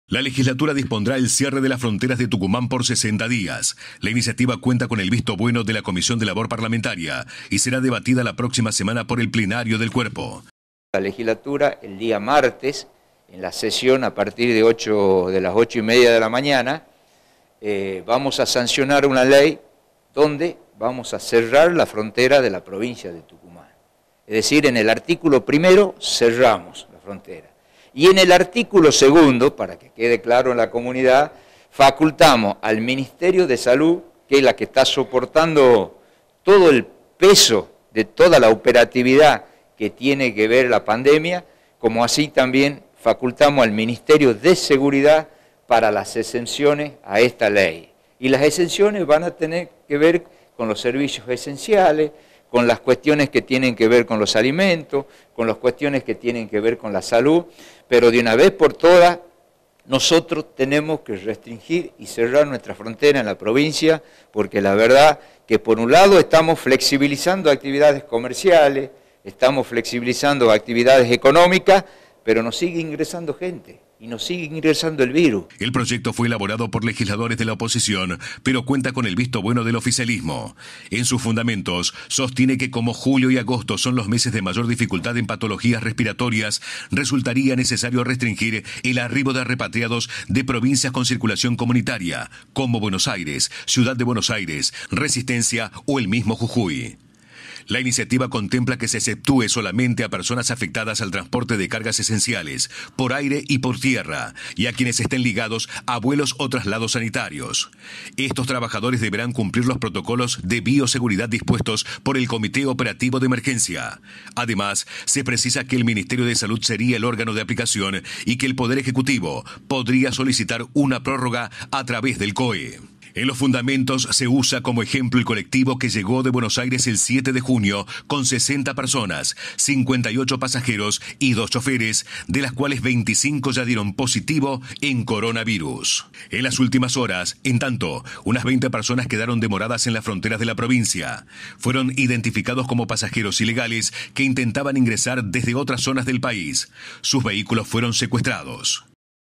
Cerrarán las fronteras de Tucumán
La Legislatura de la Provincia fijará severas circulaciones para circular.